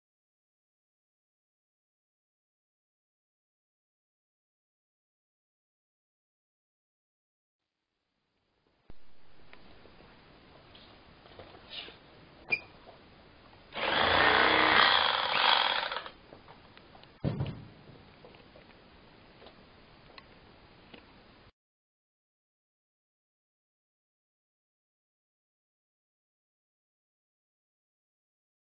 Portable Bag Sewer | Hand Held Bag Stitcher | Fischbein Model F
The Fischbein F-Series bag closers are handheld, portable, industrial bag sewing machines ideal for small volume (4-8 bags/min.) bag closing applications. Weighing only 11 lb., the handheld F-Series can easily sew multi-wall paper, polypropylene, burlap and jute bags. Available F-Series portable bag closing models include: vertical bag sewing, horizontal bag sewing and tape bag sewing.